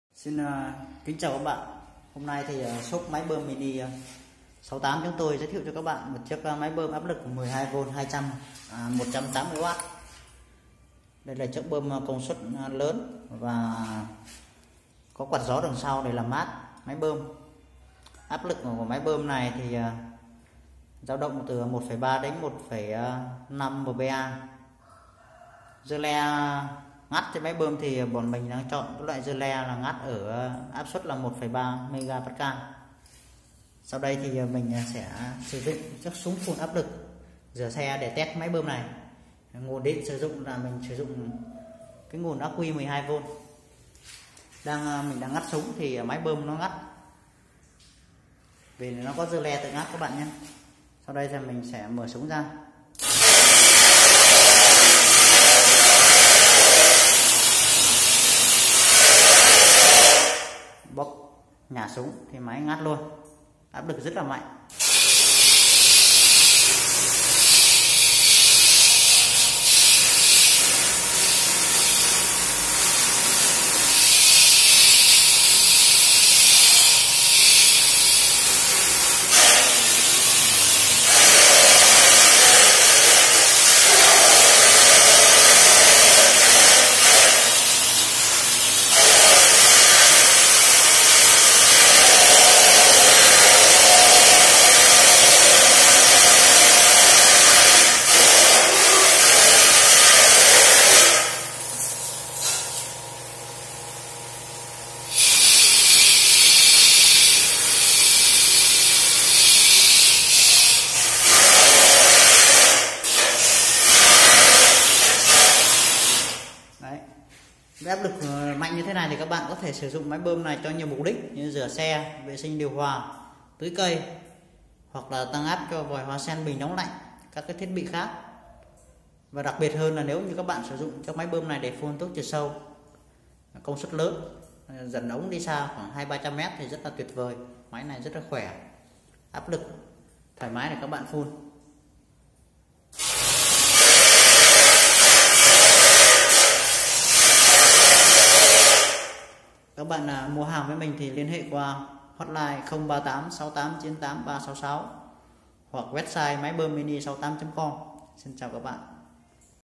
Test máy bơm áp lực 12v-180w
Máy bơm áp lực cao cấp 12v công suất 180w là dòng bơm áp lực cao nhất trên thị trường hiện nay với công suất lớn, kích thước mô tơ lớn và đặc biệt có gắn quạt gió làm mát để máy bơm hoạt động liên tục trong thời gian dài hơn.

Thông số kỹ thuật:

- Lưu lượng 15l/phút
- Áp lực hoạt động 13 bar tương đương 1,3mpa

Đặc điểm nổi bật:
- Có quạt gió làm mát
- Tự ngắt khi khóa bơm
- Đẩy xa 200-300m và hút sâu 4-8m

Ứng dụng:

- Được sử dụng như dùng rửa xe, rửa điều hòa, bơm phun thuốc sâu công suất lớn kéo với đường ống dẫn dài ở khu vực đồi núi, bơm tưới lan với hệ thống giàn lớn
- bơm đẩy cao lên nhà cao tầng, tăng áp máy giặt, hệ thống nước gia đình, bơm vệ sinh các xe đựng thực phẩm, vệ sinh chuồng chại.

Phụ kiện đi kèm: lọc, rơ le, đầu nối..
Đặc biệt sẽ được tặng 2,5m ống hút.